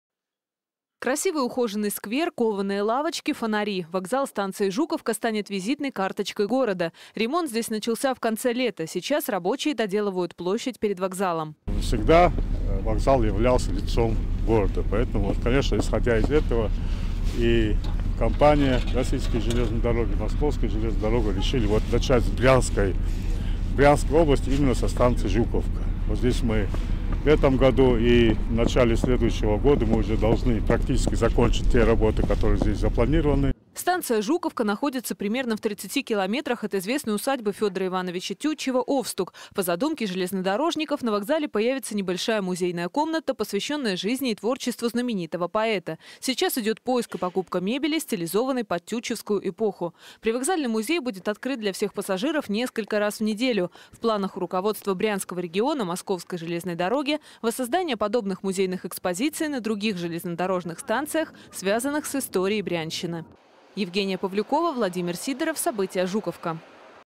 ремонтируют жуковский вокзал 08 12 16
Жуковский вокзал станет еще одной достопримечательностью региона. Несмотря на холода там не прекращаются ремонтные работы. Измениться должна и площадь перед вокзалом, и одно из внутренних помещений.